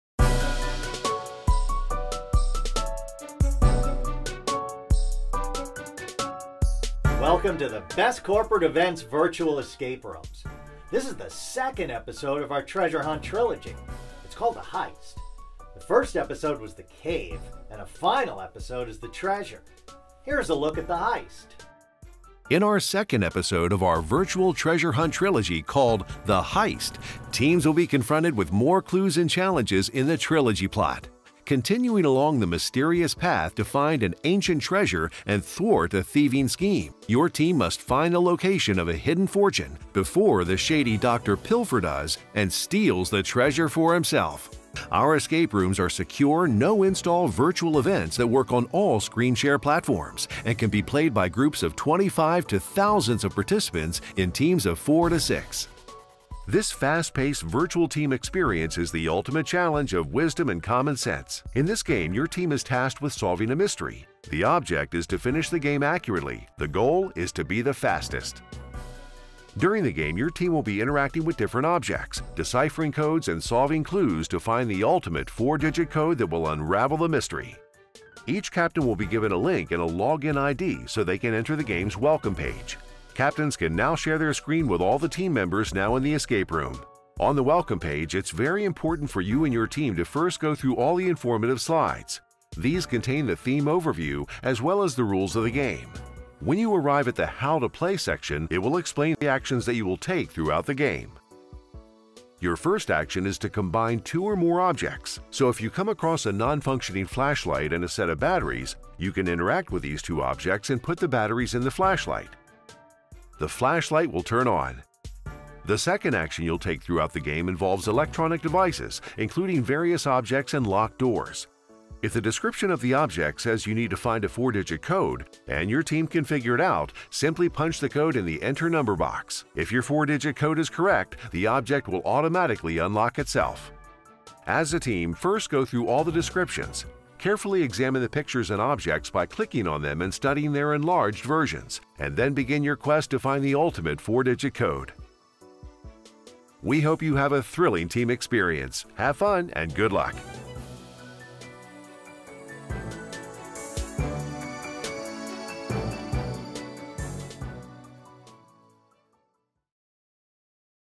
Treasure Hunt Trilogy – Episode 2: The Heist
The Heist is Episode #2 of our Virtual Treasure Hunt Trilogy where teams will be confronted with more clues and challenges in the trilogy plot. Continuing along the path to find an ancient treasure and thwart a thieving scheme, the team's character must find the answers to the location of a hidden treasure before the shady Dr. Pilfer does and steals the treasure for himself. With an immersive, continuing storyline, clues of varying types and difficulty, detailed graphics, and an advanced escape room platform, this program will intrigue, challenge, and bond your team.